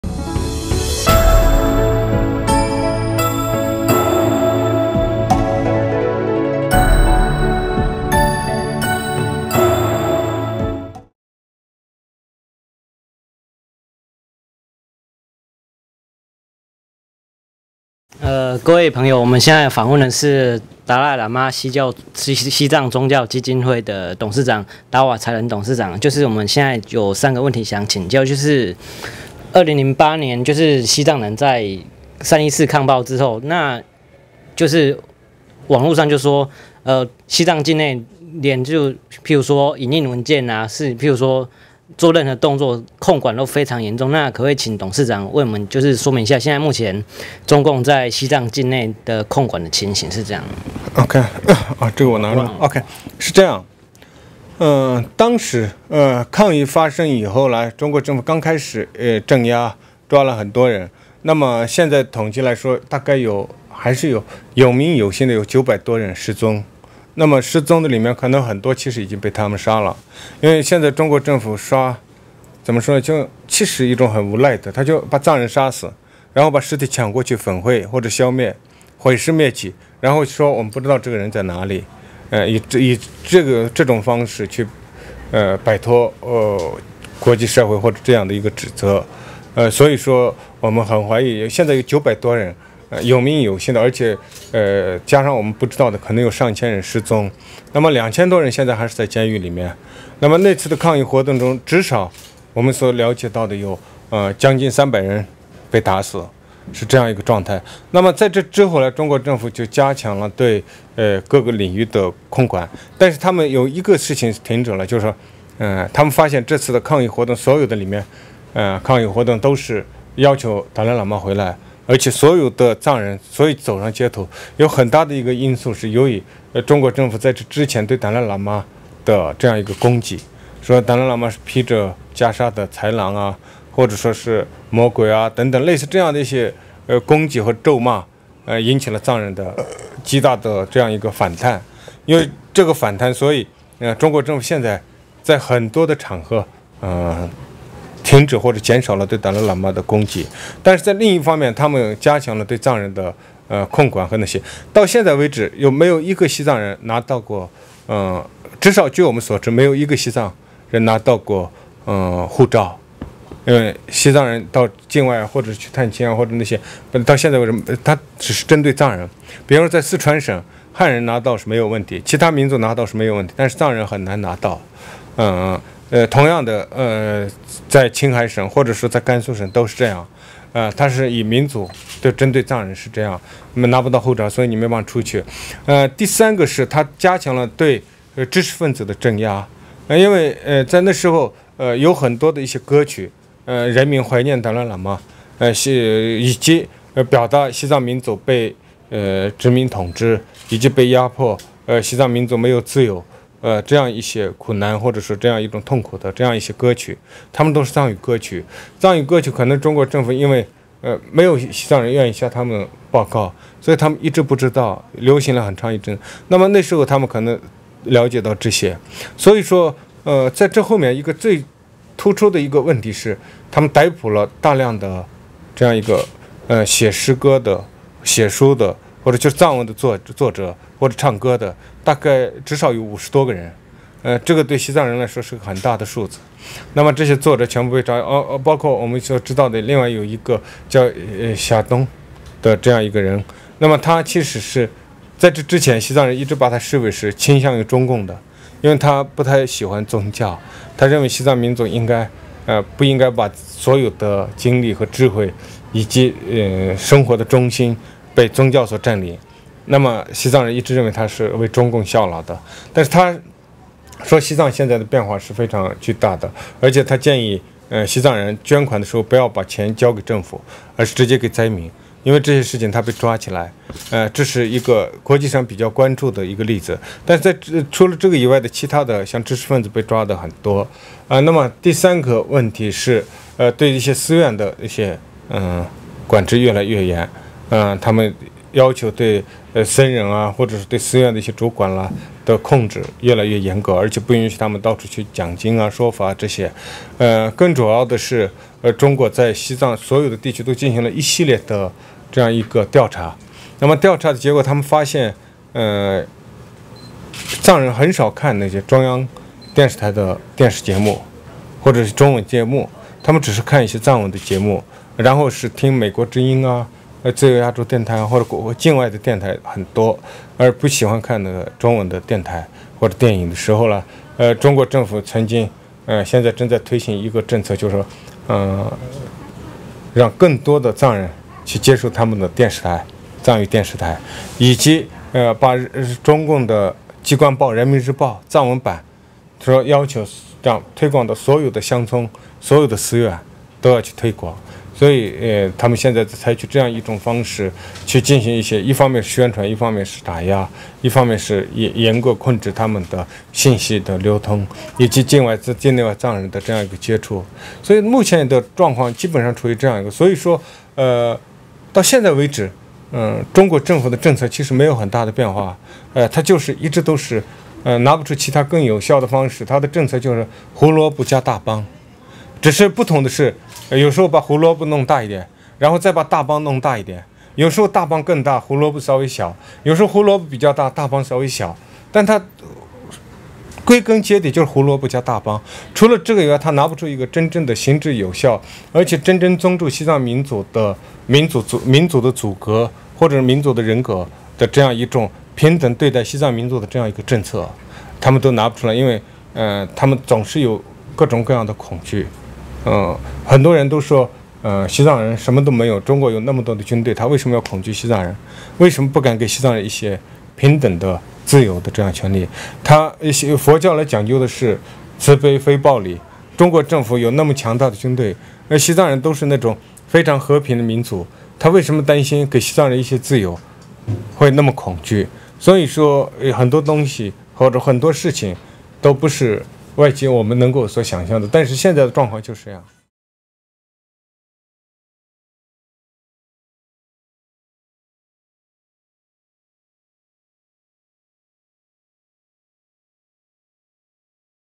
01/05 達瓦才仁專訪@大地 20101002
達賴喇嘛西藏宗教基金會董事長達瓦才仁接受台灣大地文教基金會的專訪

 Q1. 2008年3月14日的抗暴行動之後，中共如何對圖博進行更嚴密的管控？現在那邊的情況如何？
Q2. 對於因為爭取圖博人自治與人權而遭中共逮捕的民主人士我們該如何幫助他們？例如紀錄片「無畏」的製片當知項欠。
Q3. 台灣人民如何跟圖博人合作確保自由民主人權？
Q4. 對於在台藏人抗議故宮文物展的看法。
Q5. 跟達賴喇嘛尊者見面與互動的感覺？